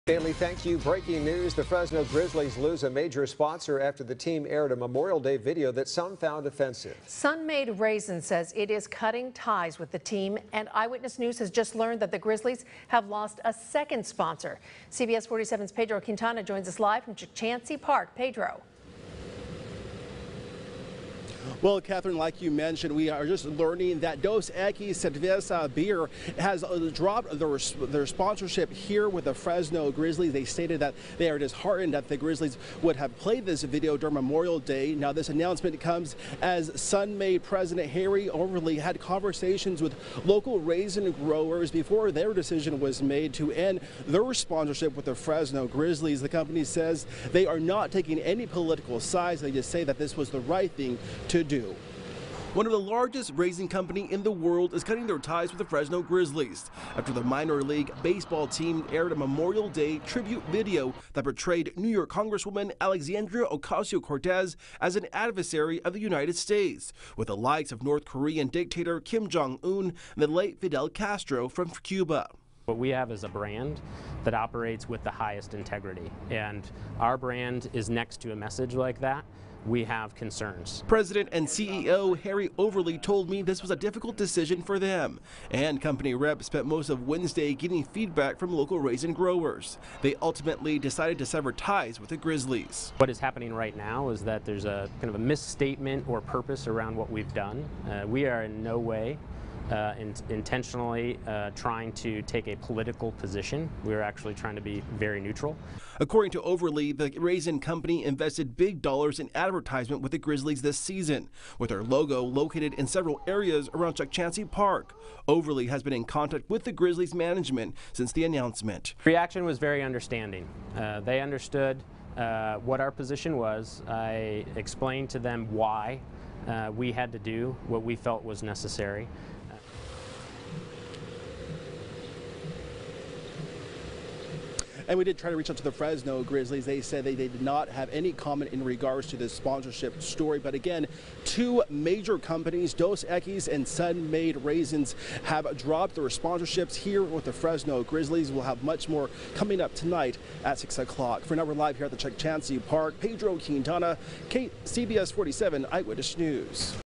Sun-Maid, Dos Equis drops Fresno Grizzlies sponsorship amid Memorial Day video controversy
Sun-Maid and Dos Equis announced Thursday they are cutting ties with the Fresno Grizzlies after the minor league baseball team aired a Memorial Day video, which some found offensive.

"What we have is a brand that operates with the highest integrity and our brand is next to a message like that, we have concerns," Sun-Maid President and CEO Harry Overly said.

On Memorial Day, the Grizzlies aired a Memorial Day tribute video that portrayed New York Congresswoman Alexandra Ocasio-Cortez as an adversary of the United States with the like of North Korean Dictator Kim Jong Un and the late Fidel Castro from Cuba.

"What is happening right now is that there is some kind of misstatement or purpose around what we've done. We are in no way intentionally trying to take a political position, we are actually trying to be very neutral," he said.

Overly said this was a difficult decision for the company. He said he and his team spent most of Wednesday getting feedback from local raisin growers, they ultimately decided to sever ties with the Grizzlies.